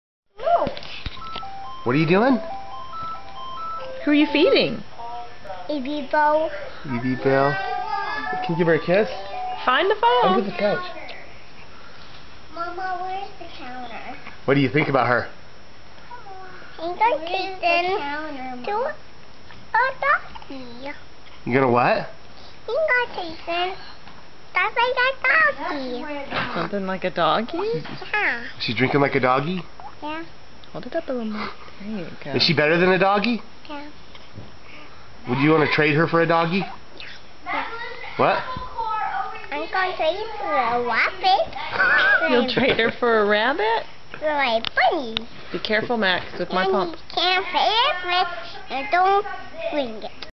Eli wouldn't trade her for a doggie, but...
Elijah feeding Evie Belle and talking about how she might change into a dog.  he wouldn't trade her for a dog but...